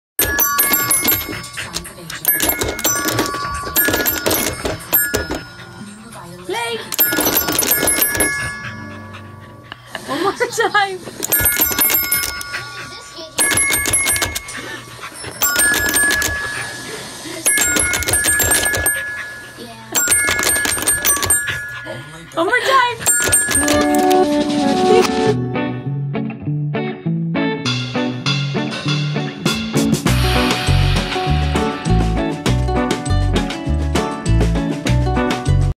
Cute Corgi Dog Loves Playing the Piano
This cute corgi loves playing the piano with her puffy paws.